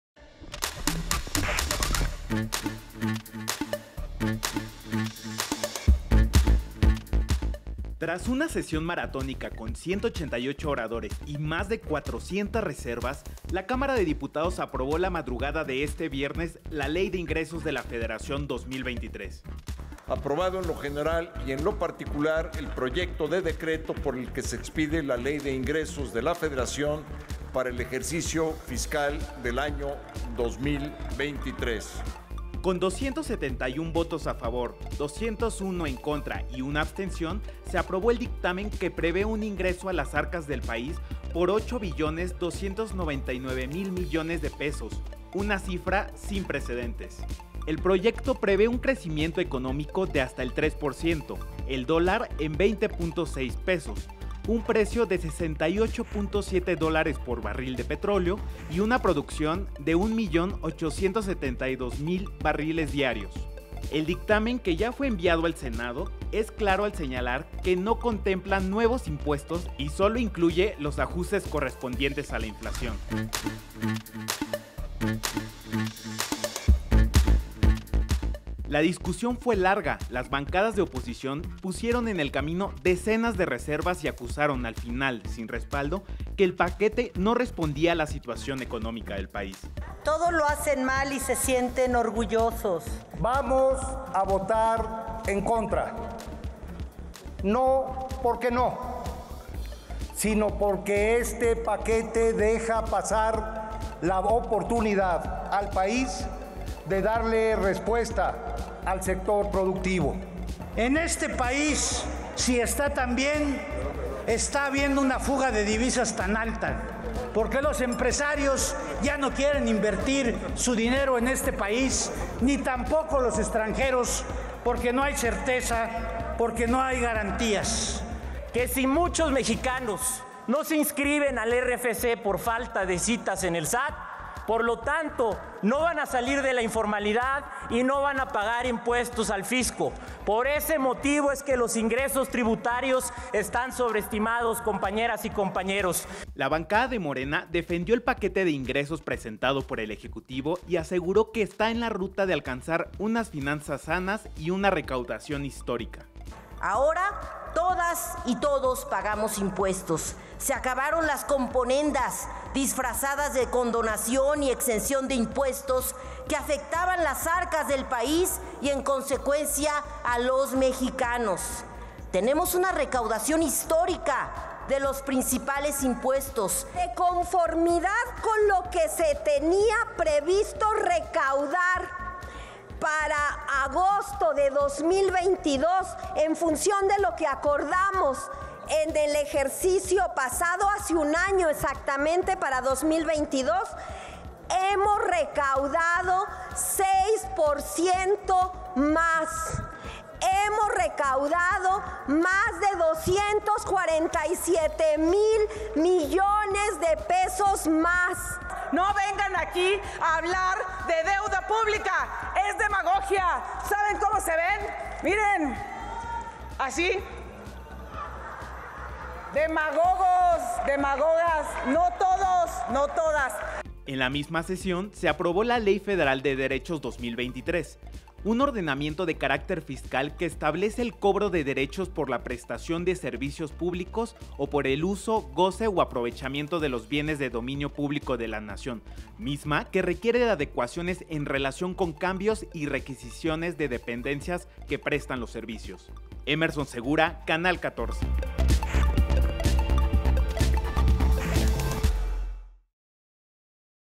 La Cámara de Diputados aprobó la Ley de Ingresos 2023
Con 271 votos a favor y 201 en contra, la Cámara de Diputados aprobó la Ley de Ingresos que prevé que la Federación perciba 8 billones de pesos, y un crecimiento económico de hasta el 3%.